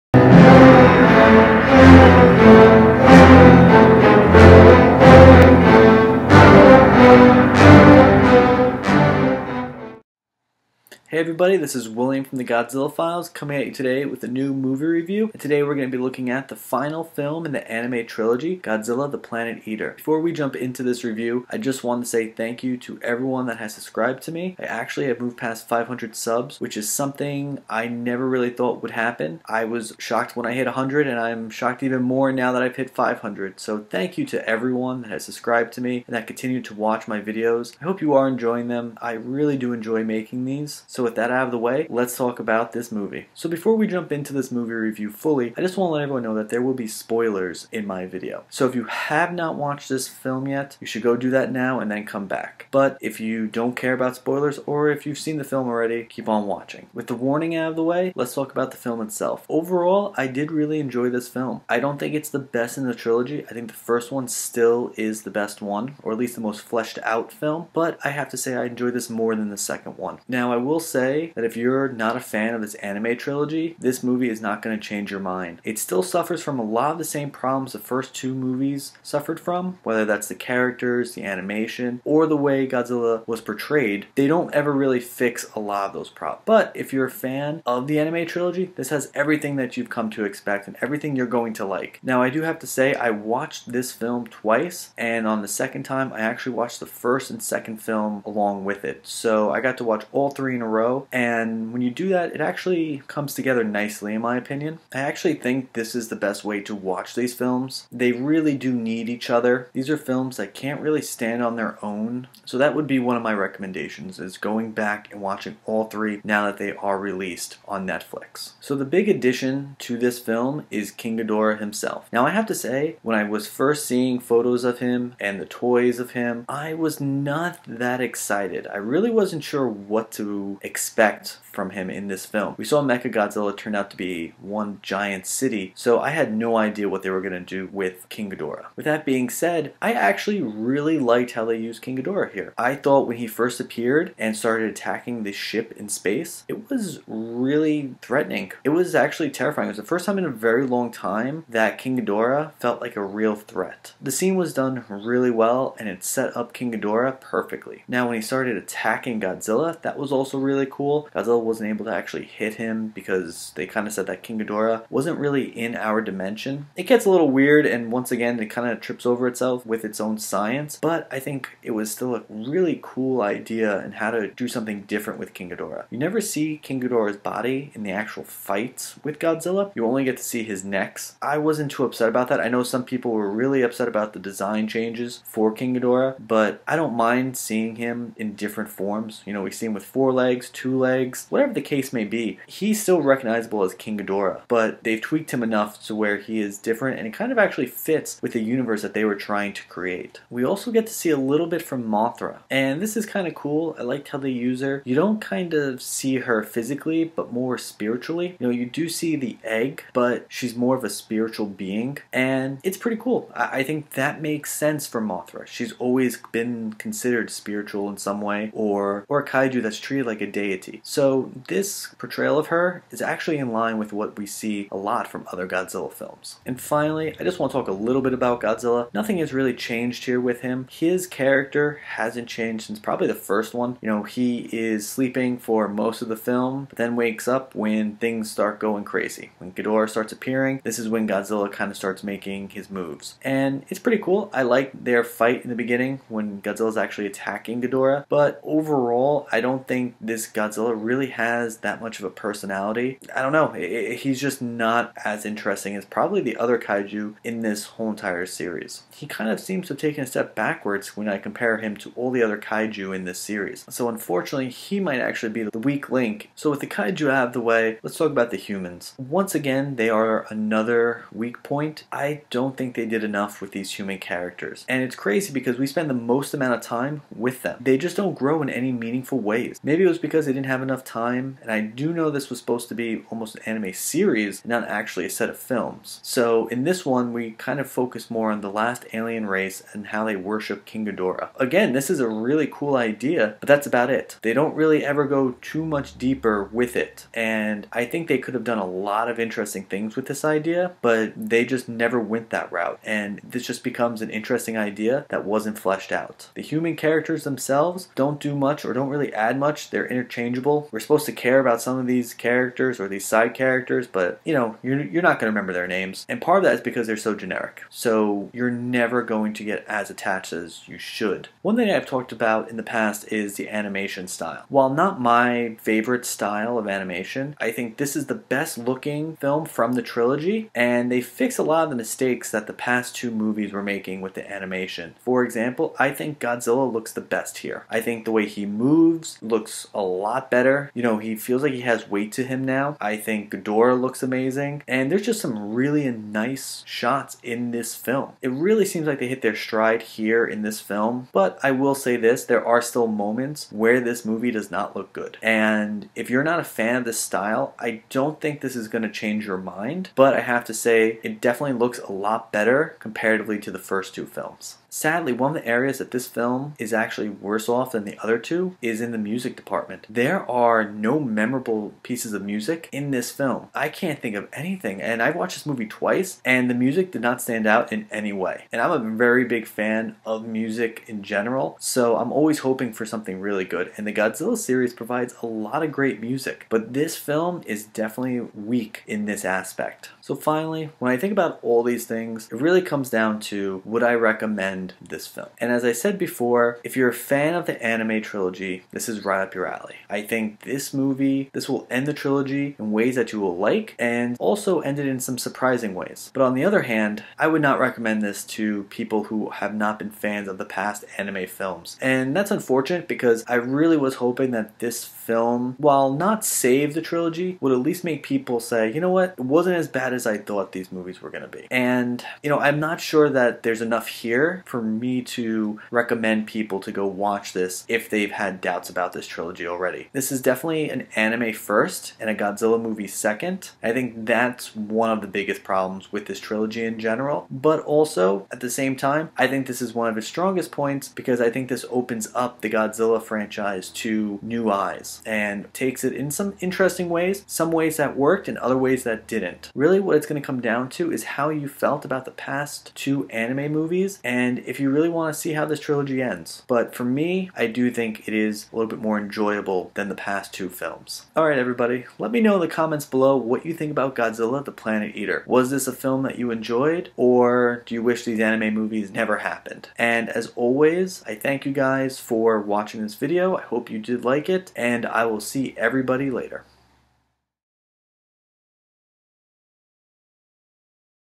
Godzilla The Planet Eater Review
I look at the final film in the Godzilla anime trilogy, Godzilla The Planet Eater